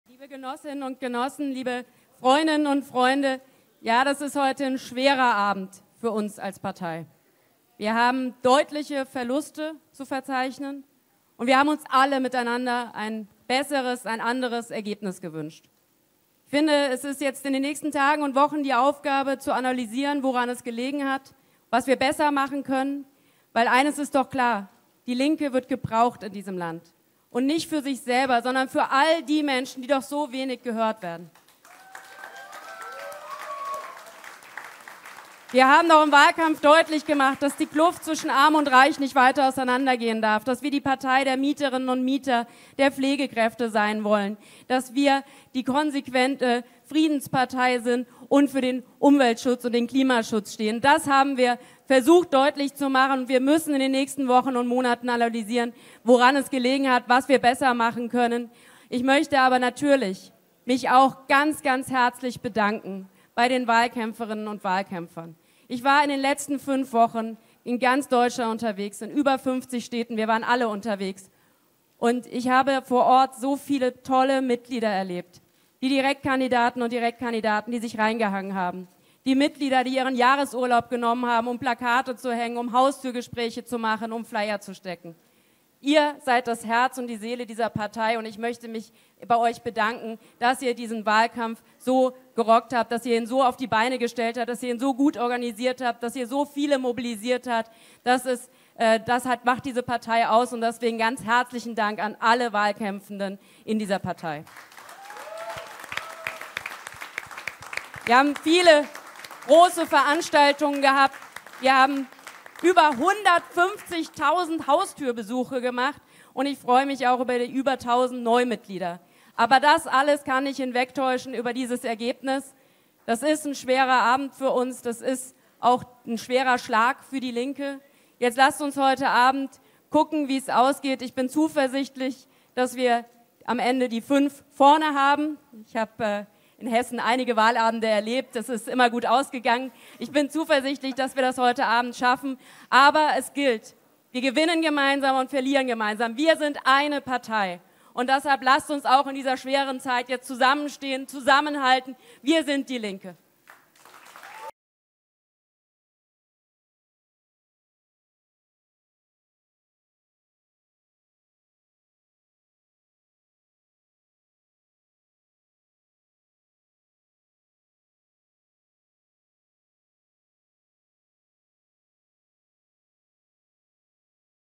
NIEDERLAGE bei den LINKEN: Janine Wissler ist zuversichtlich, dass die Linke im Bundestag bleibt
Die jüngste ZDF-Hochrechnung zeigt die SPD bei 25,6 Prozent und die Union bei 24,4 Prozent. Demnach kommen die Grünen auf 14,7 Prozent, die FDP auf 11,6 Prozent, die AfD auf 10,3 Prozent und die Linke auf 5,0 Prozent.

Die Linke muss weiter um ihren Platz im Bundestag bangen. Parteivorsitzende Janine Wissler ist zuversichtlich, dass die Linke es schaffen wird.